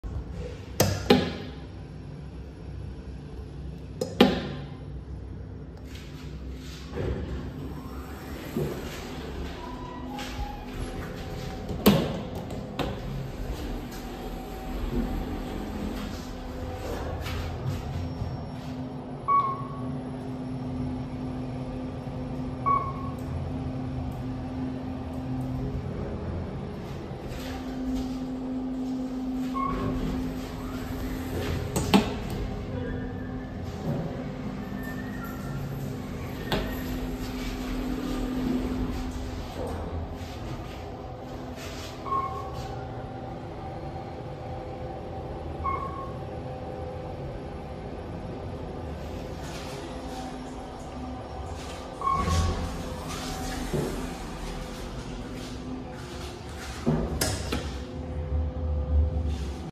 Nice 2019 KONE Ecodisc Traction Elevator 1 | West Beach Parkade | White Rock, BC
This was a pretty nice kone. My. First KSS 140

Taken day before Upload